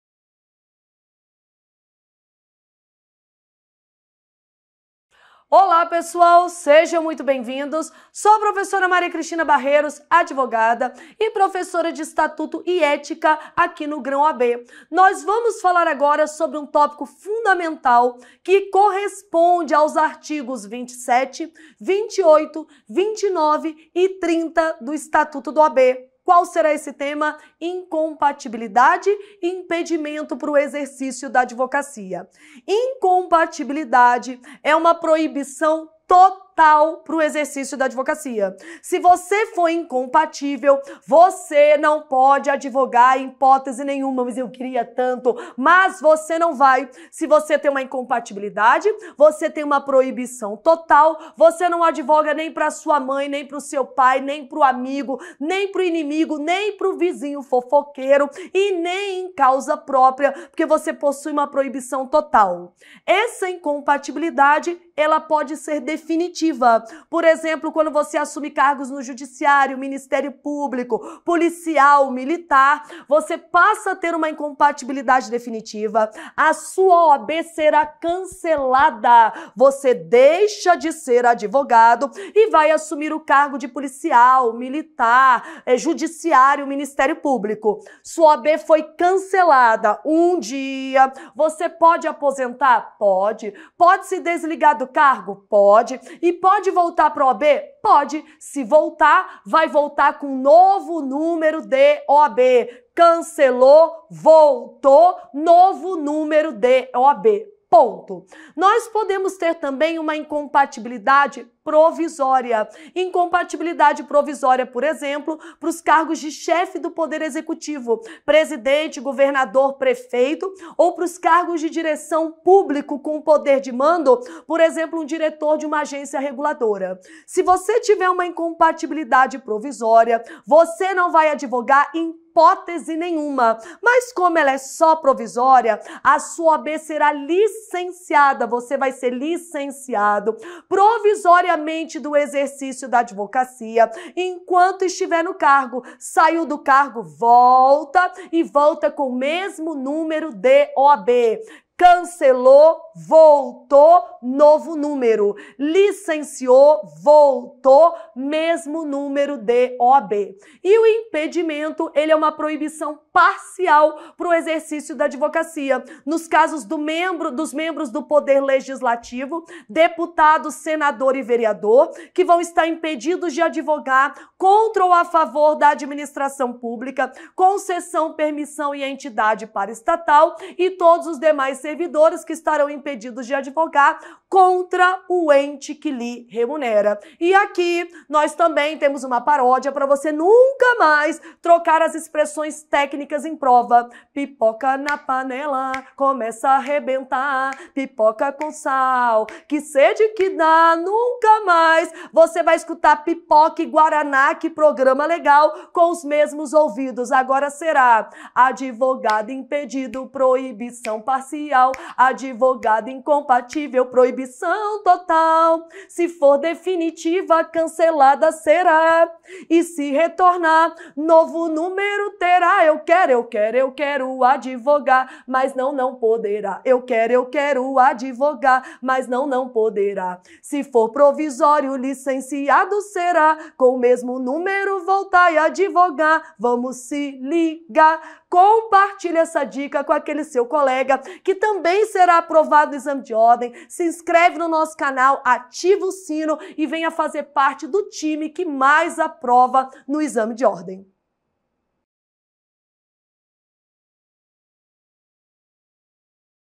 OAB Express: Dica sobre Incompatibilidade e Impedimento para a 1ª fase do Exame da OAB!
Neste vídeo, a professora Maria Christina trará dicas pra você entender e acertar questões sobre Incompatibilidade e Impedimento para a 1ª fase do Exame da OAB.